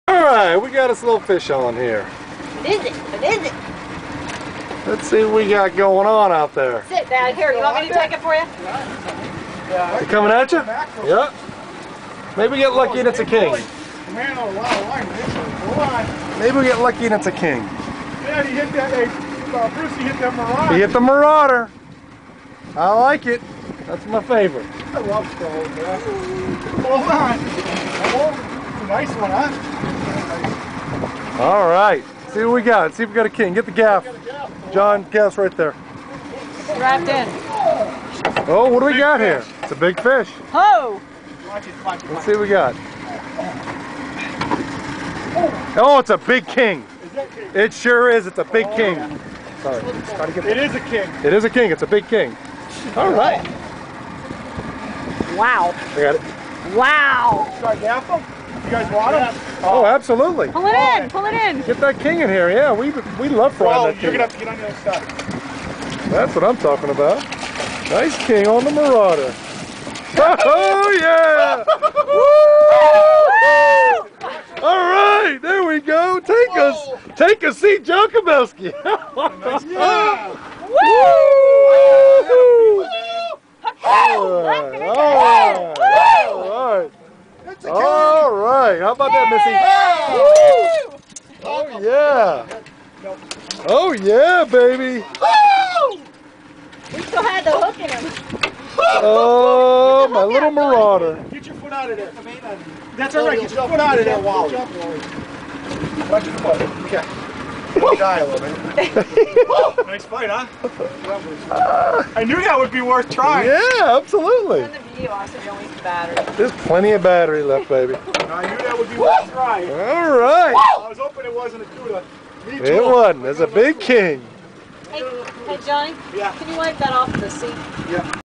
PC King Fishing with Mr Kobelsky April 05.mpg
John Kobelsky's Dad hooks into a nice King Mackerel and gets very excited when we bring it on the boat.  We were fishing on Hookem & Cookem out of Port Canaveral in April 2005.